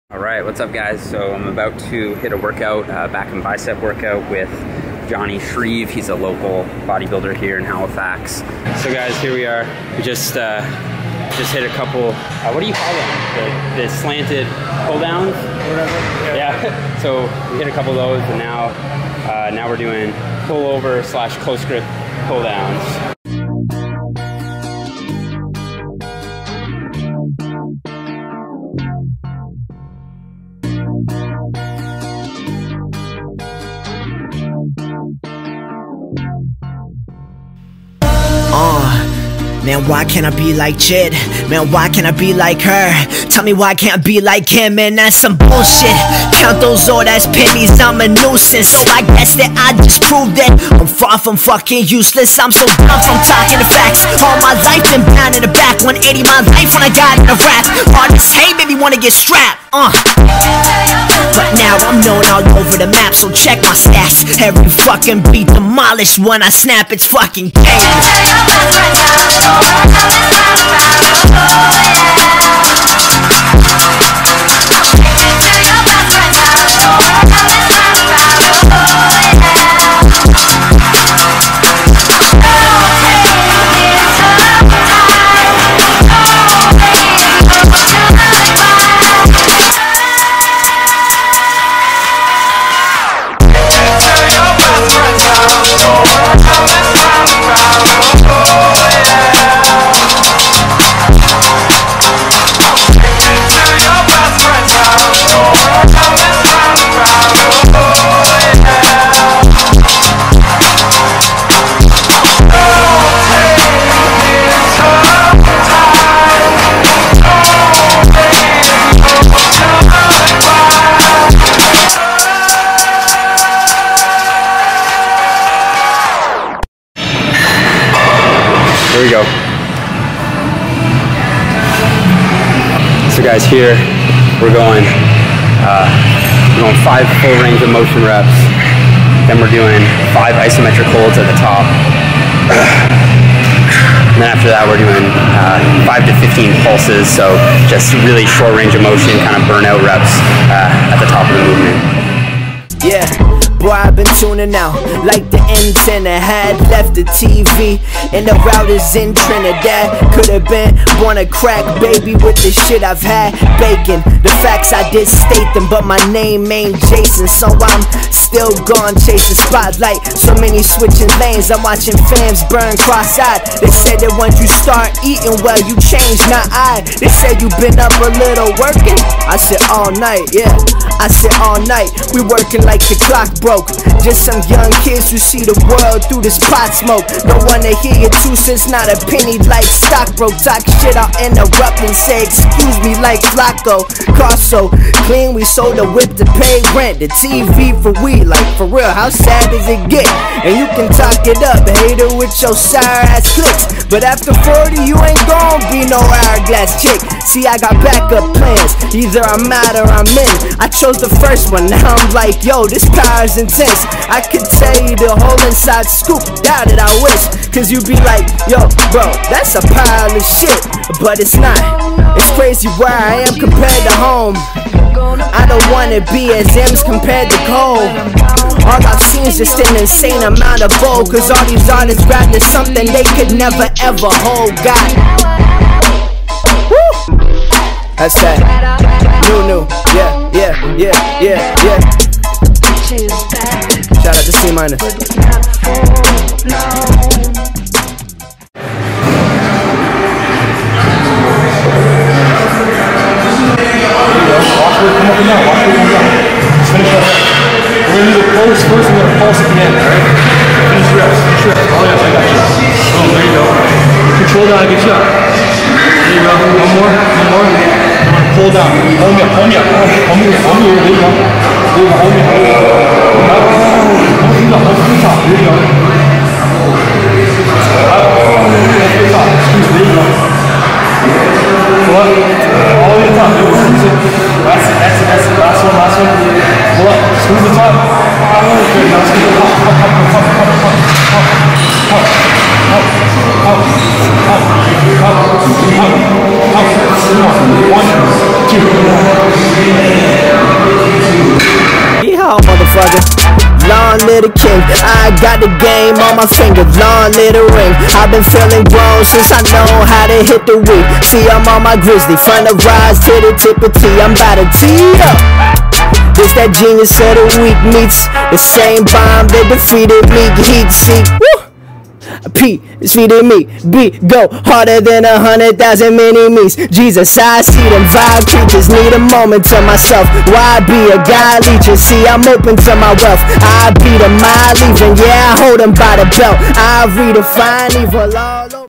Full Y3T Bodybuilding Back and Biceps Routine ft. Jonni Shreve
FOR CONTEST PREP/ COACHING from Robin